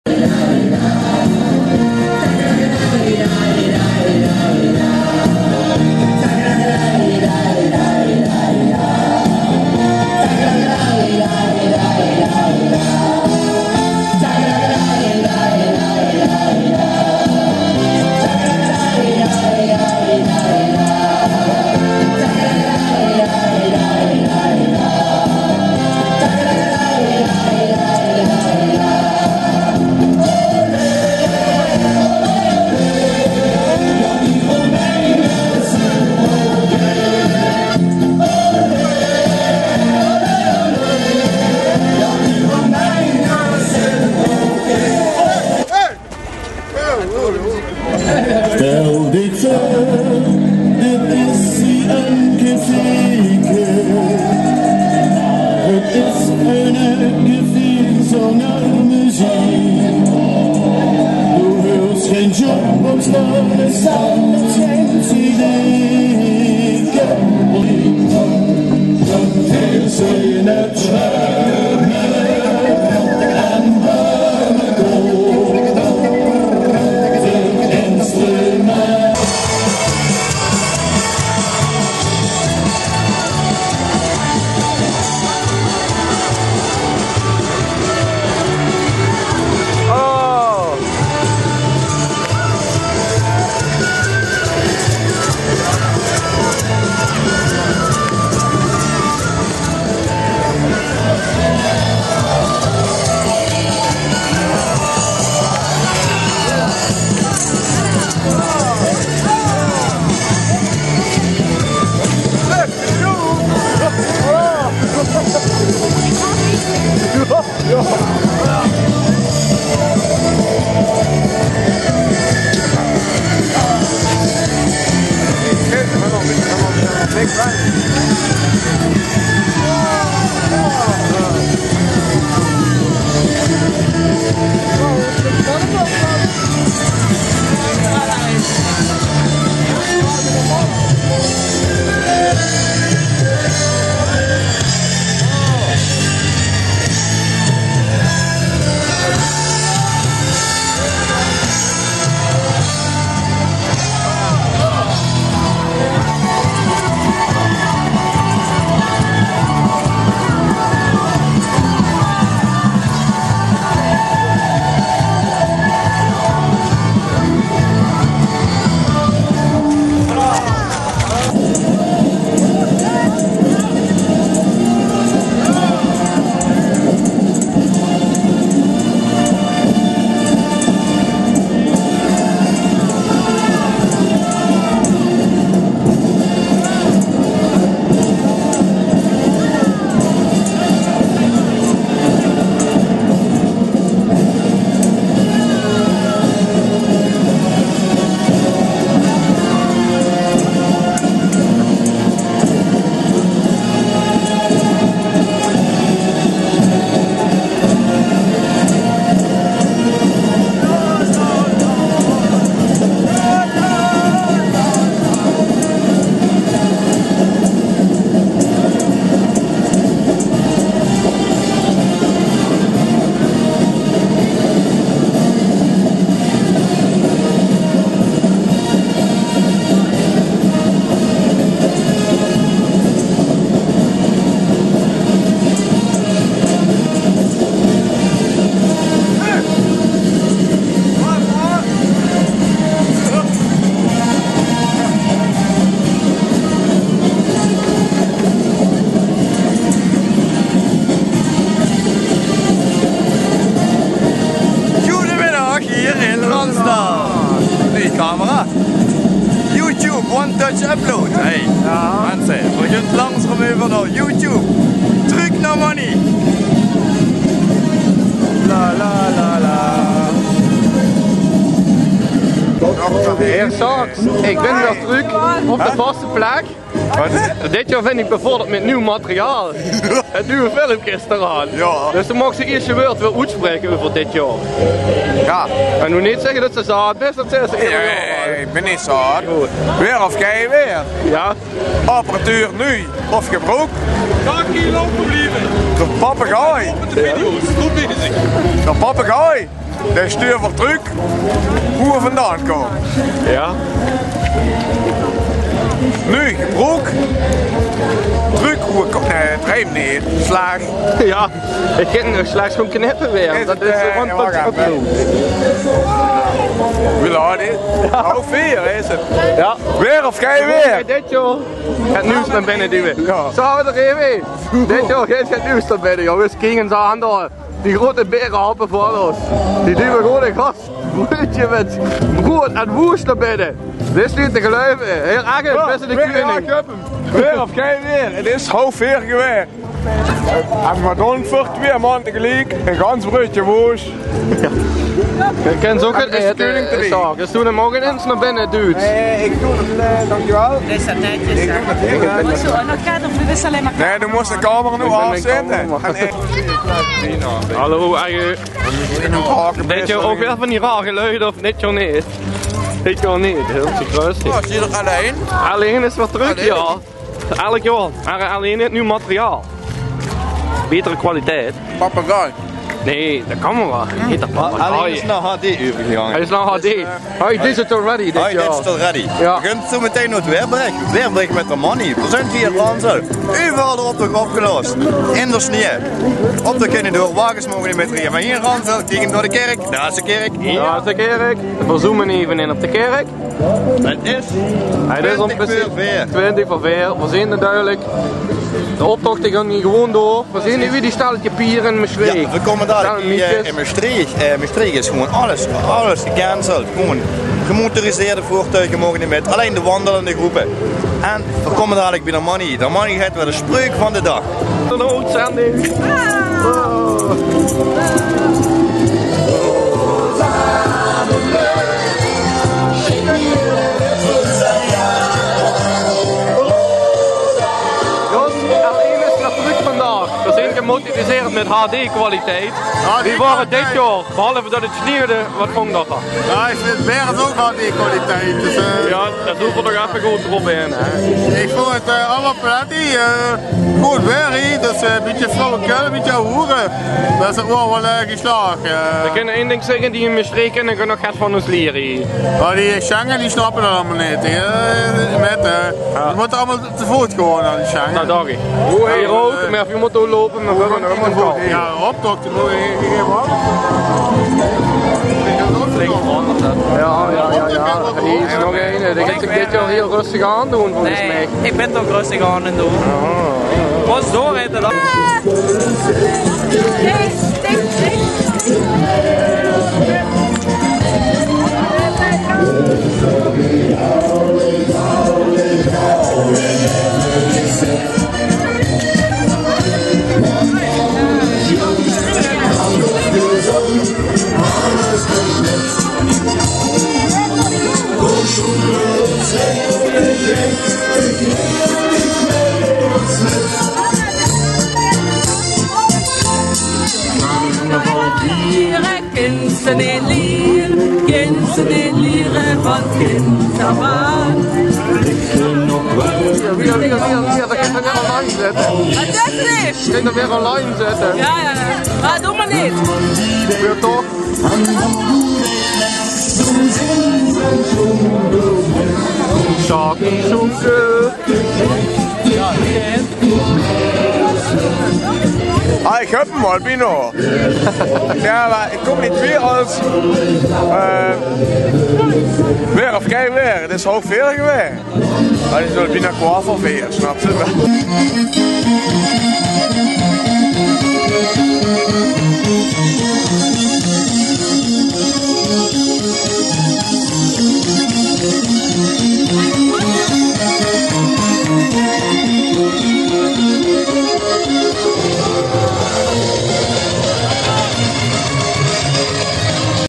Breuzelaere 2010 part1
Deel 1 van de aftermovie.
Carnaval 2010 van de Breuzelaere uit Ransdaal